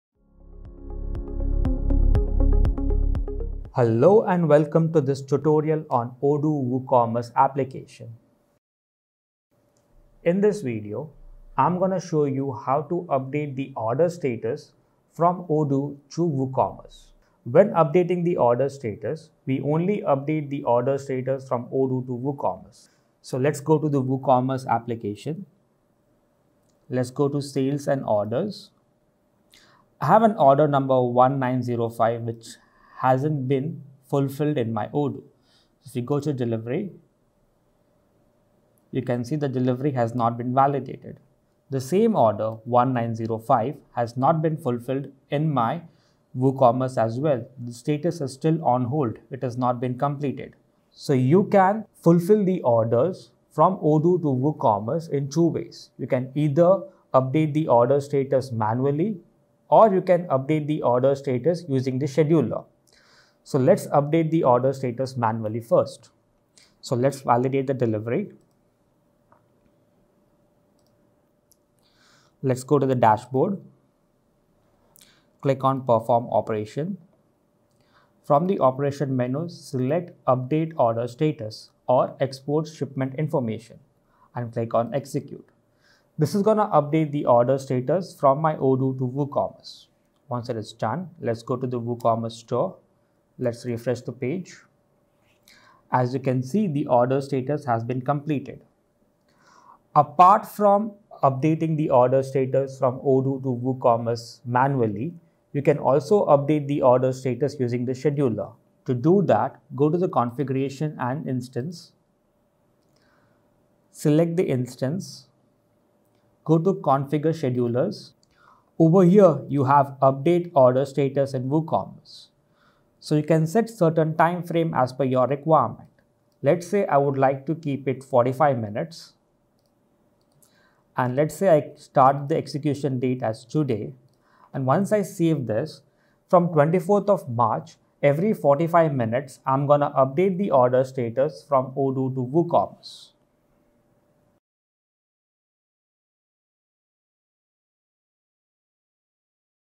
Update Order Status | Odoo WooCommerce Connector
In this video, we will discuss once the order is imported to Odoo and how to update the order status on WooCommerce.

Already purchased Odoo WooCommerce Connector and need support? Connect with Emipro Help Desk from the below link: